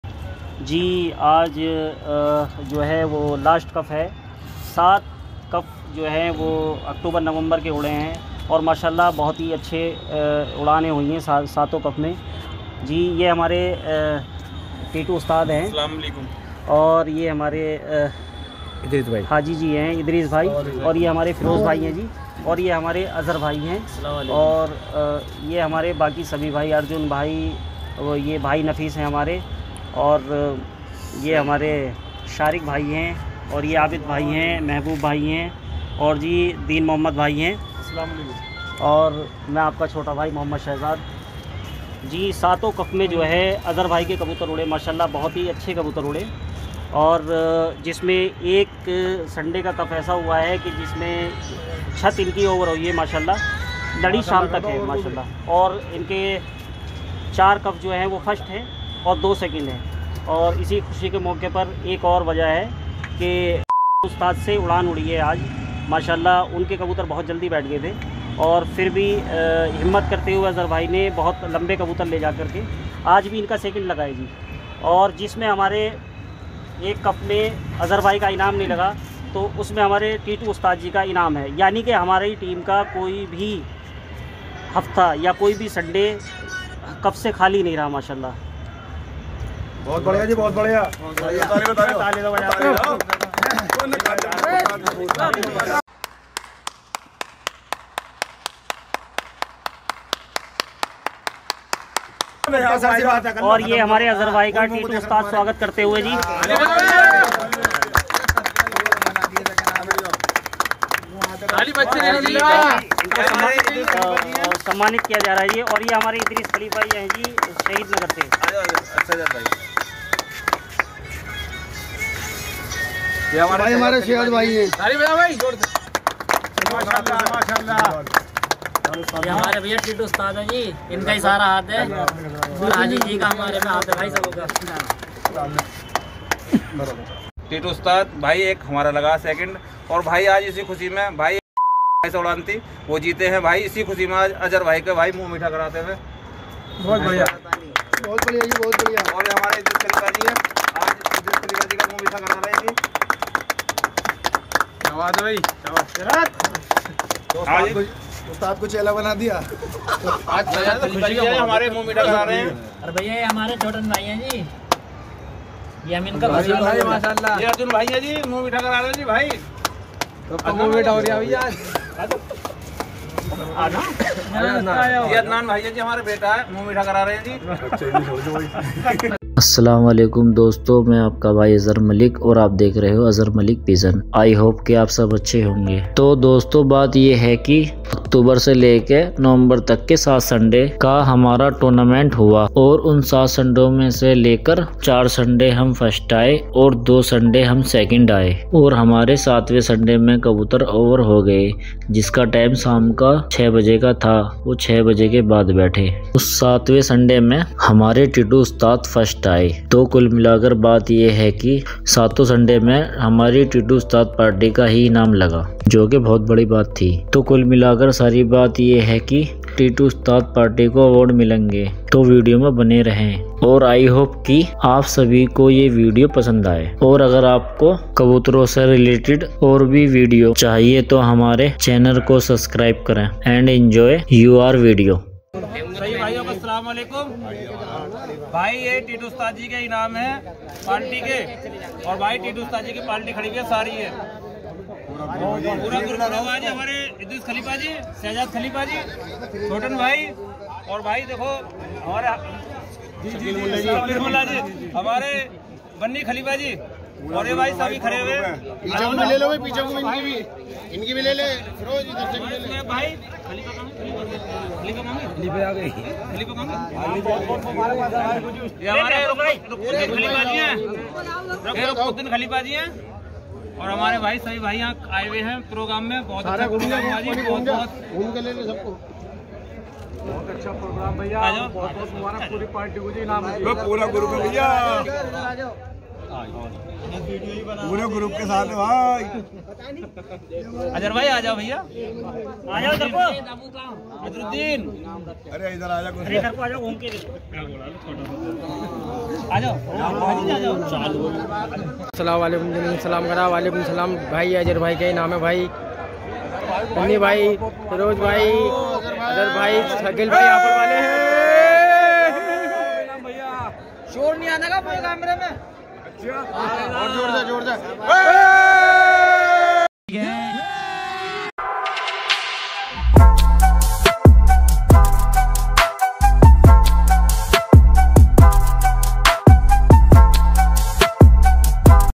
Tournament Ki Tayyari 😎🏆 ||7 Hafto Mai 7 Sunday ki race🏆 🕊 || 7 Sunday on 7 week pigeons race🕊
Aur inshallah jald hi agli race mai live ayenge aur agar aapko hamari  race live dekhni hai to subscrib kardo and bell icon bhi daba dena ta ki aap stream miss na kar de.